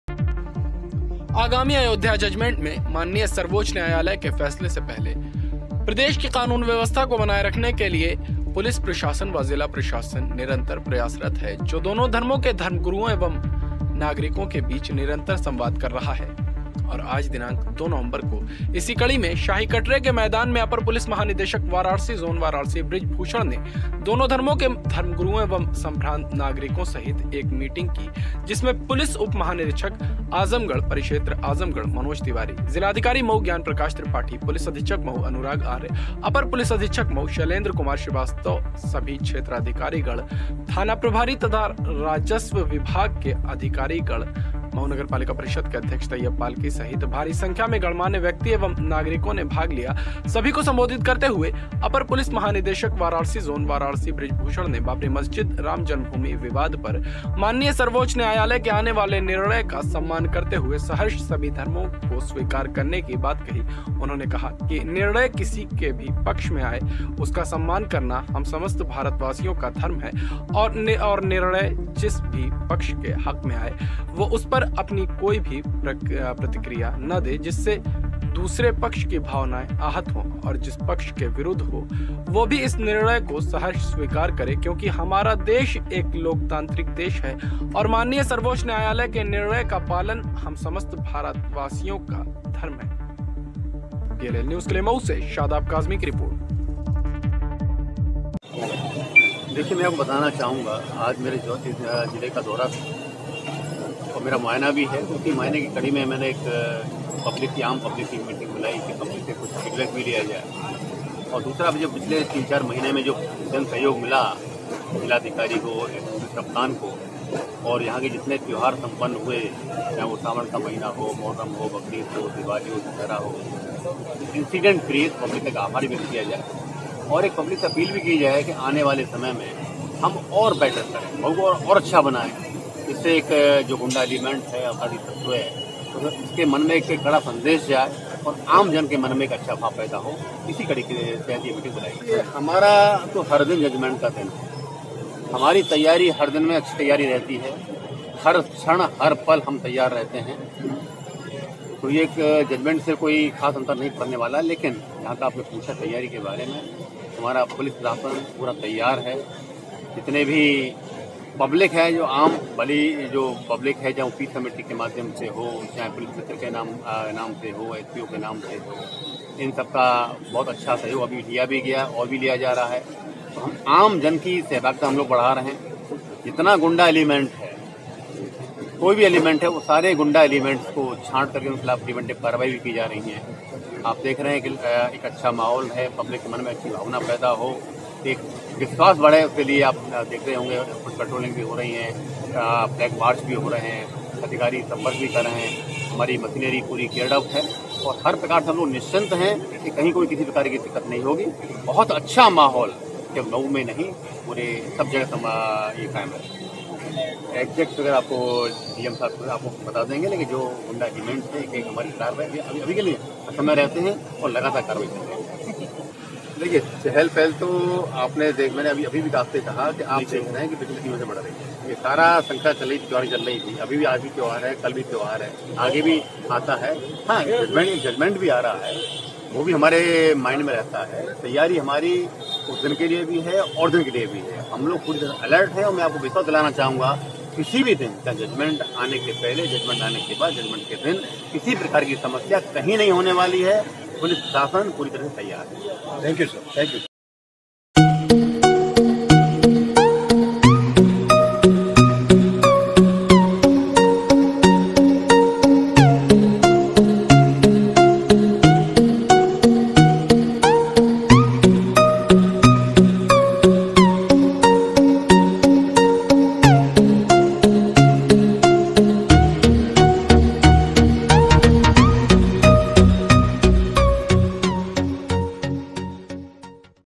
बाबरी मस्जिद/राम जन्म भूमि निर्णय से पहले प्रदेश सरकार सहित ज़िला प्रशासन अलर्ट || PLL News
रिपोर्ट - शादाब काज़मी ( प्रबन्ध सम्पादक )